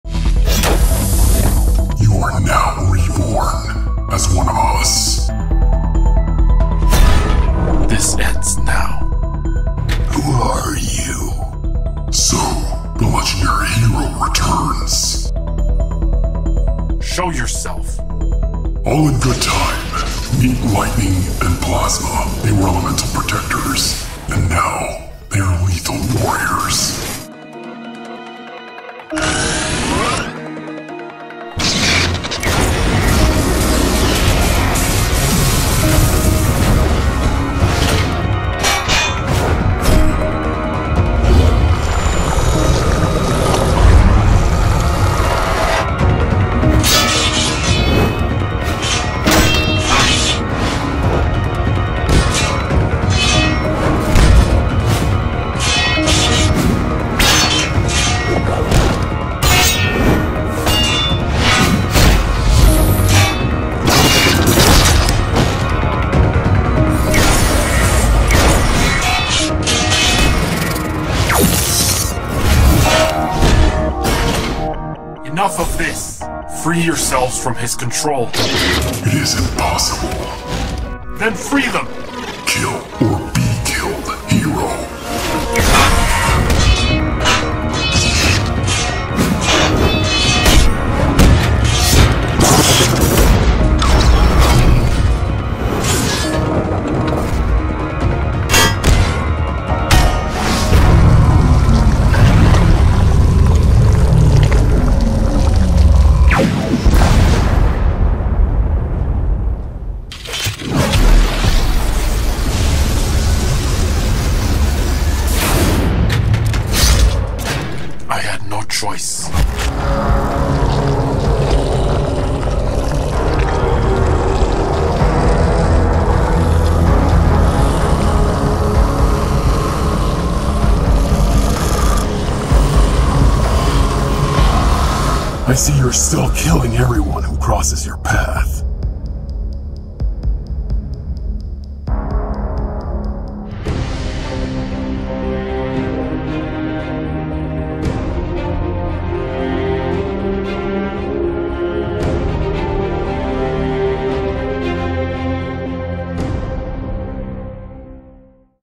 Bionicle Battles 3 - Matoro vs. Lightning & Plasma - The Toa of Ice Returns
10 years after Bionicle The Storm of Fatality, Matoro, Toa of Ice, enters the arena where Lightning and Plasma have been fighting. But he may have found his match in the new elemental warriors and their relentless onslaught. An epic battle begins in this Bionicle animation.
This Bionicle animation was created from scratch entirely by me. All models were created and rigged by me. All lighting, animation, sound design and effects as well.